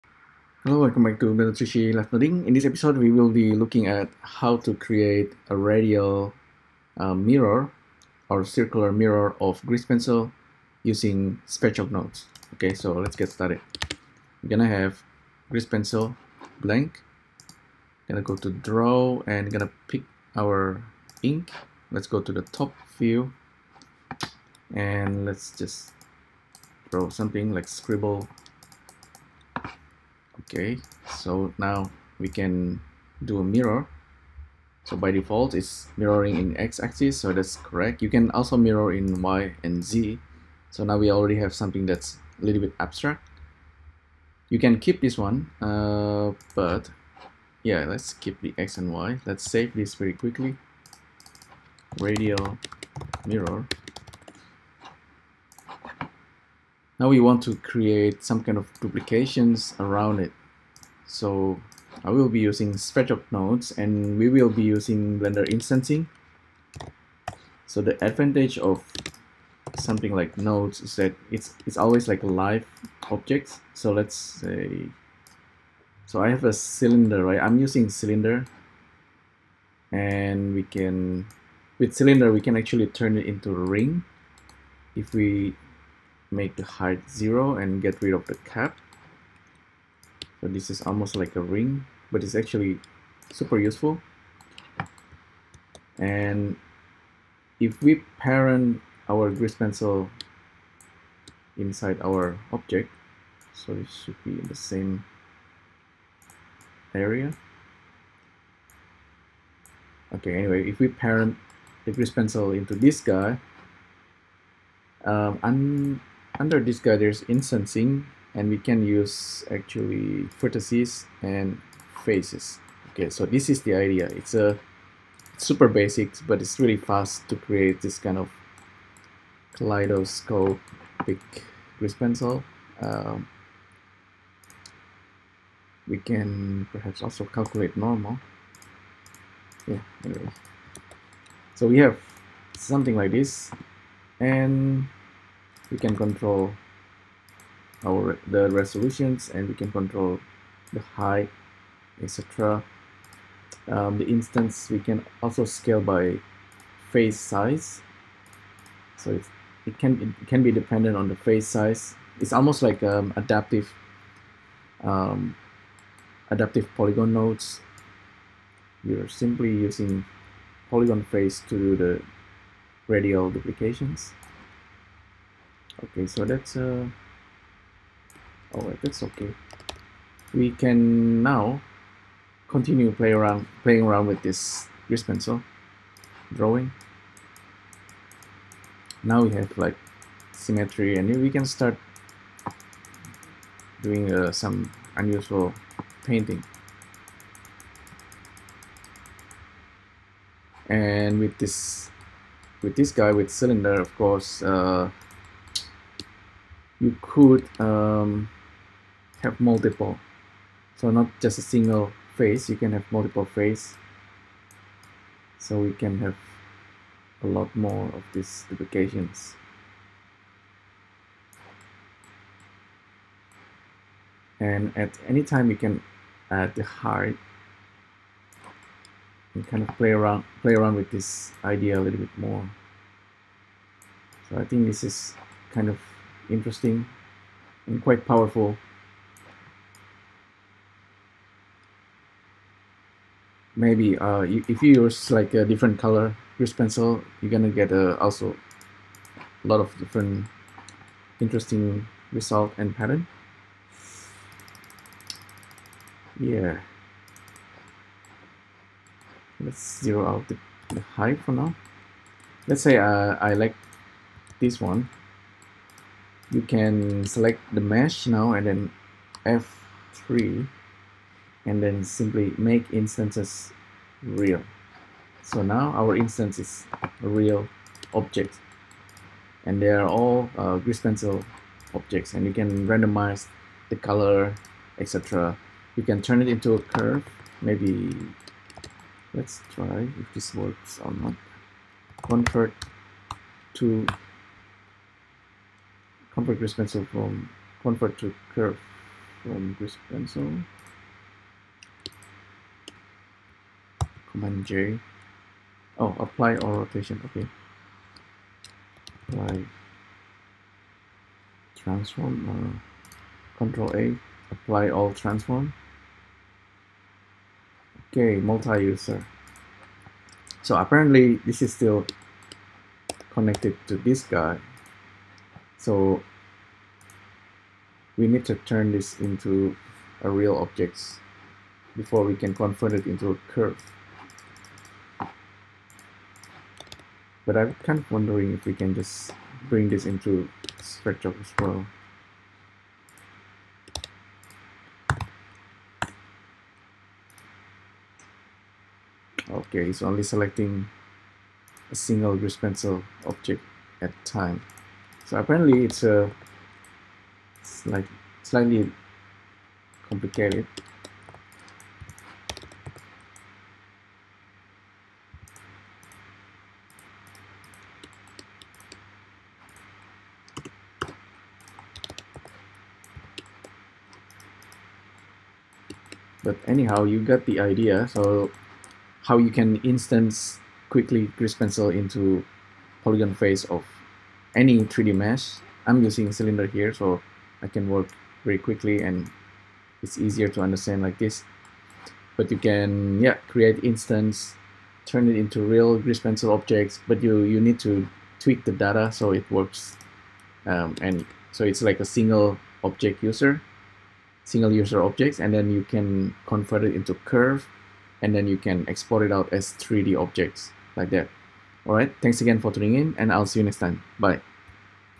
LIVENODING Grease Pencil Radial Symmetry Using SV Nodes
In this episode, I am using the instancing feature of Blender, together with SV nodes to make radial duplications of Grease Pencil objects.

I suggest you to grease pencil doodle while listening to zenful music, to have a whole blendful meditational experience.